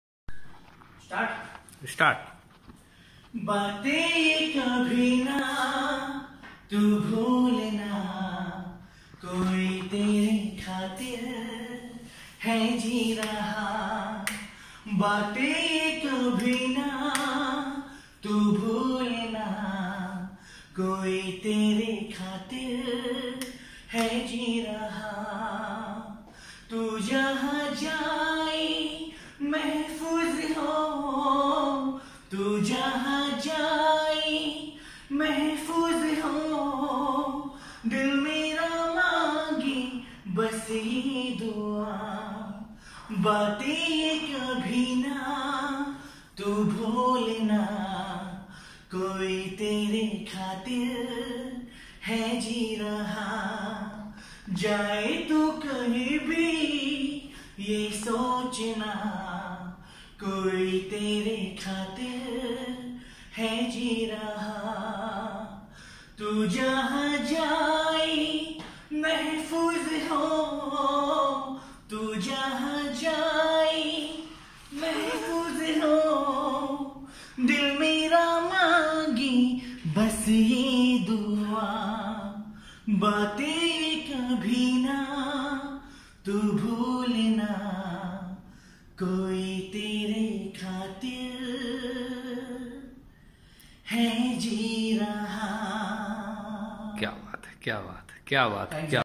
Ranu mandal ki hi tarah Nai aawaj plz listen the voice
Sohan Singh is a good artist and singer also please support him